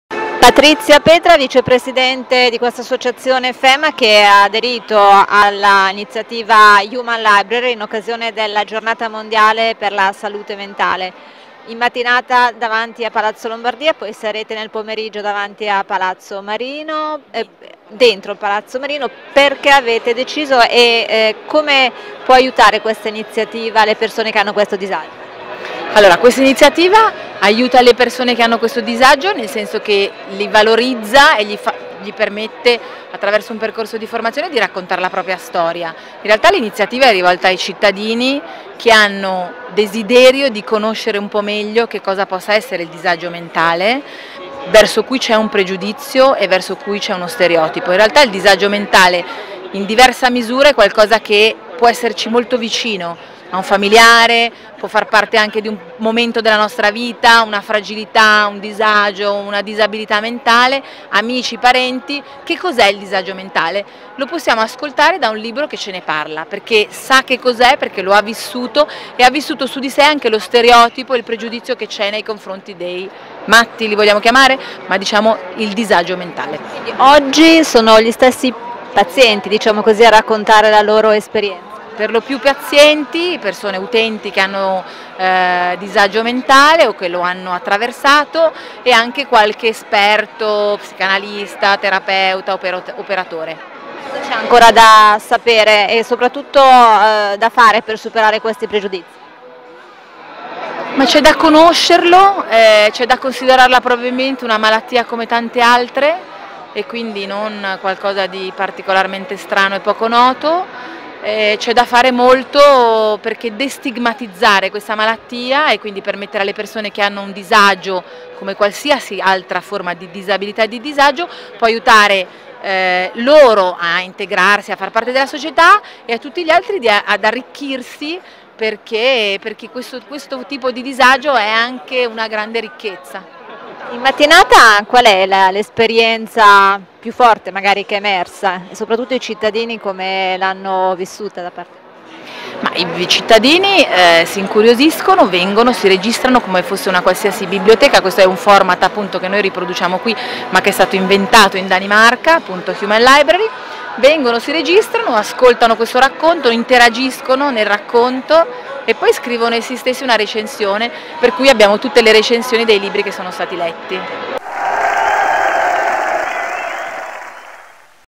Giornata della salute mentale, ecco Human Library, la libreria vivente. Intervista a Petra Mezzetti
In occasione della Giornata della salute mentale la Fondazione Empatia Milano (FEM) ha dato vita a un'iniziativa decisamente originale, una “Human Library”: una biblioteca di venti libri vivi (uomini e donne libro in carne e ossa) a disposizione del pubblico per altrettante recensioni personali.
"Human Library"  era presente la mattina del 10 ottobre in piazza Città di Lombardia davanti al Palazzo della Regione, mentre al pomeriggio si è trasferita al Comune di Milano, ospite di Palazzo Marino in sala Alessi.